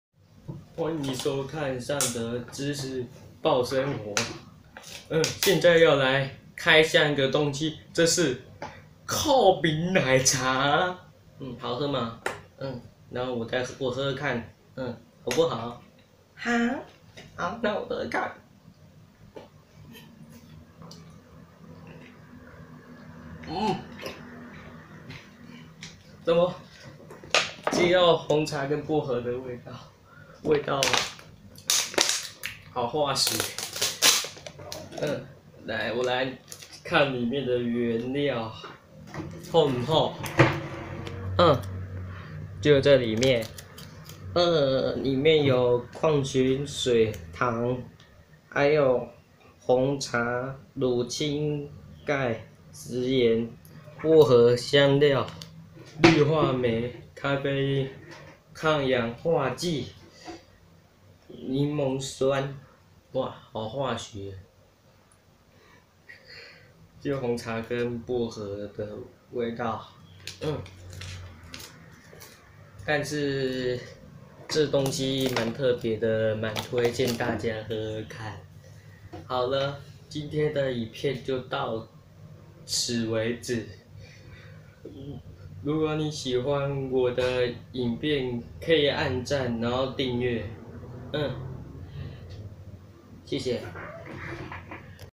尚責知識抱生活 開箱透明奶茶
{兩年後2019年新奶茶開箱影片推薦觀賞}
---開箱義美奶茶[尚責知識台-抱生活]

*開箱透明奶茶
*原物料介紹

這是本系列的第一部影片，讓你生活更充實。